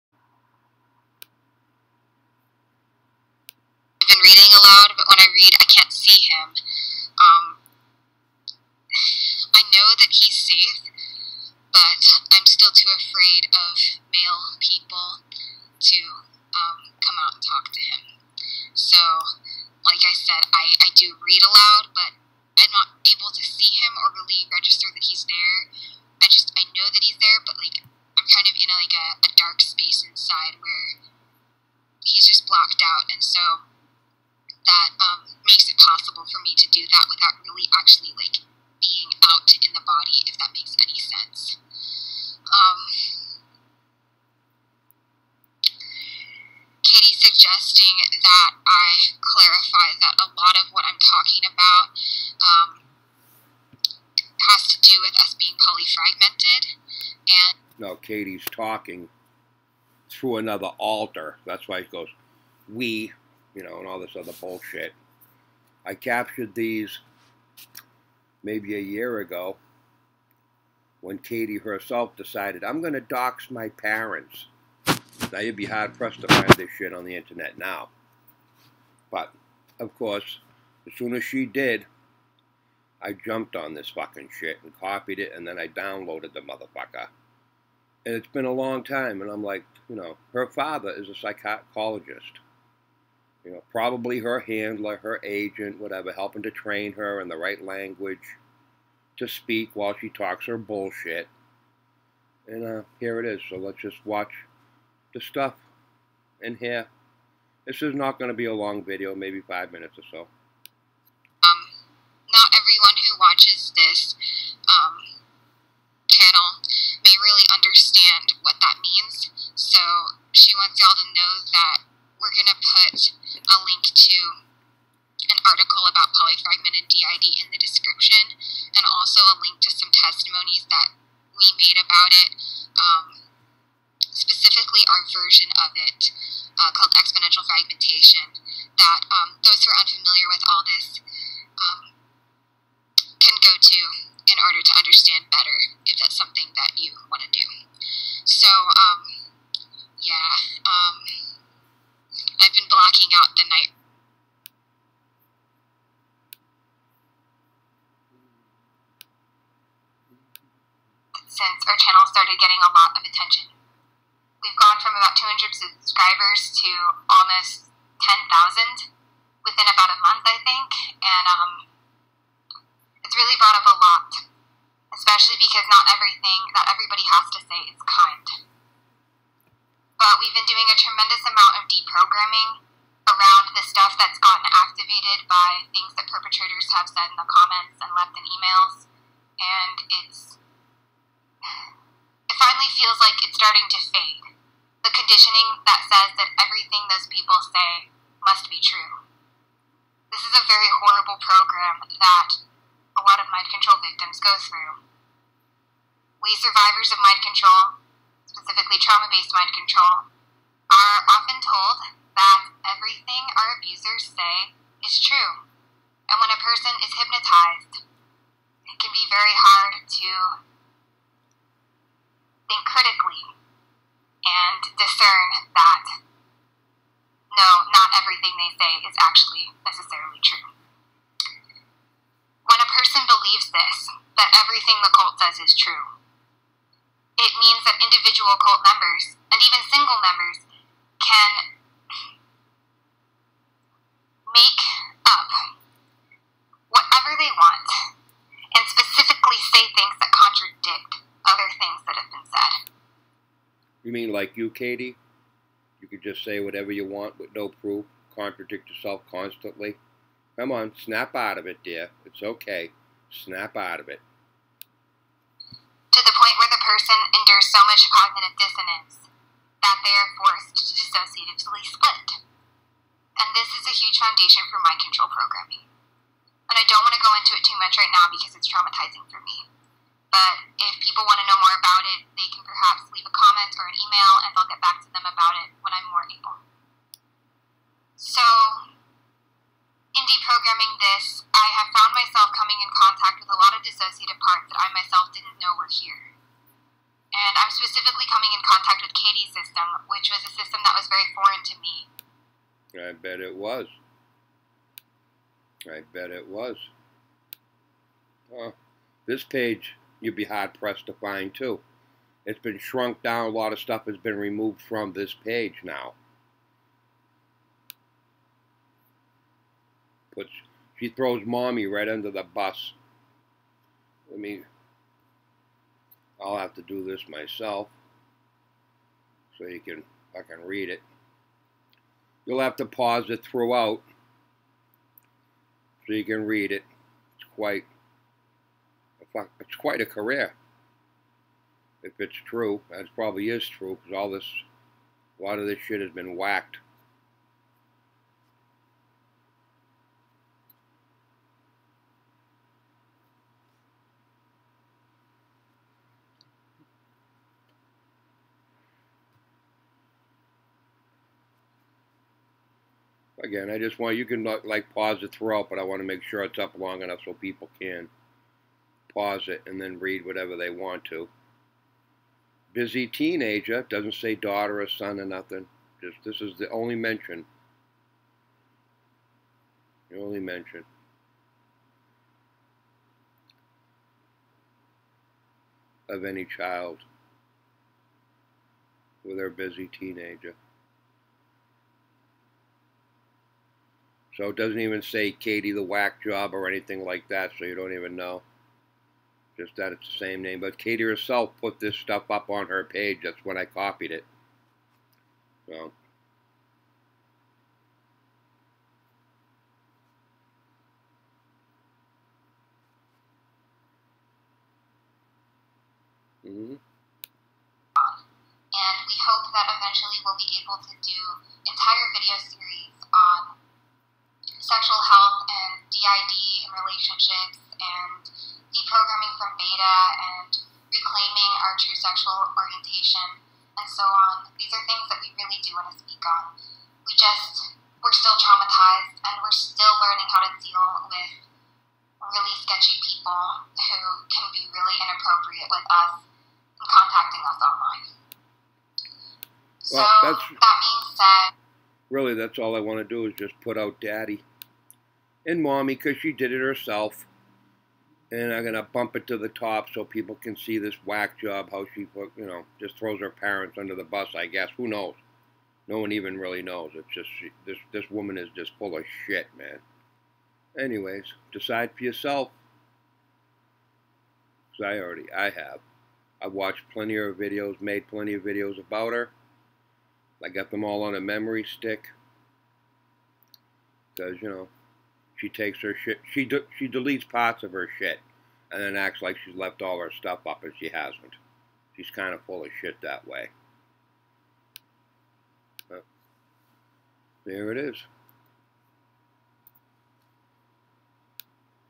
Katy Groves Doxes her parents. Copied from August 2018
Katy Groves Doxed her parents on August 2018. I copied the links she provided immediately and then I recorded what was on those pages. The psychological therapy page was removed about her father. Her mothers page was altered, by whom I don't know. But here is the pages as Katy originally posted.